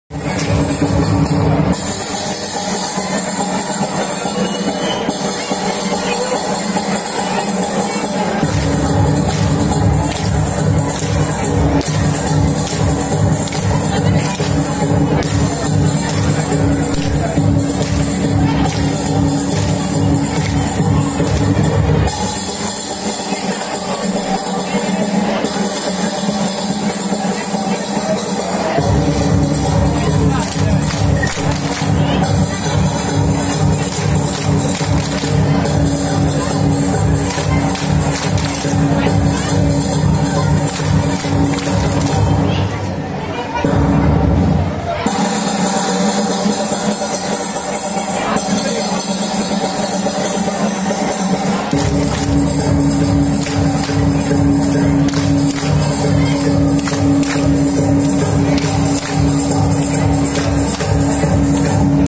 Junior School Annual Sports|বার্ষিক ক্রীড়া অনুষ্ঠান |Bangladeshi school 🇧🇩
Junior school annual sports program, Bangladesh